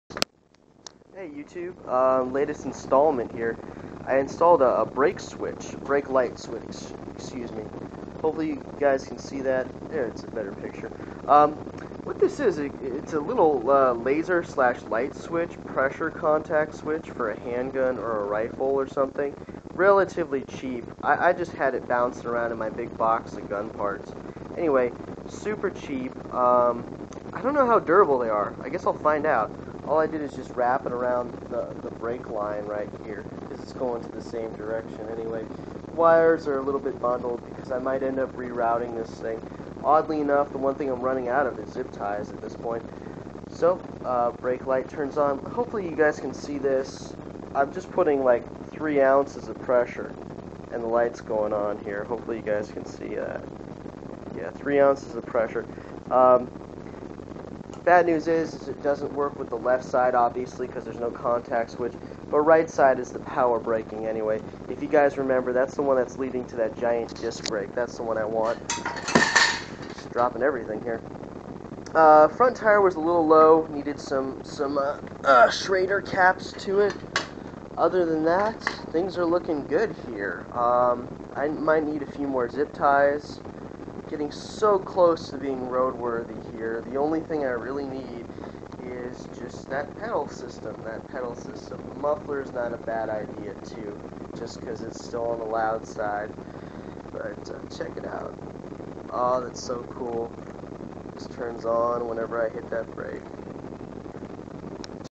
brake lights
I installed brake lights with parts I had lying around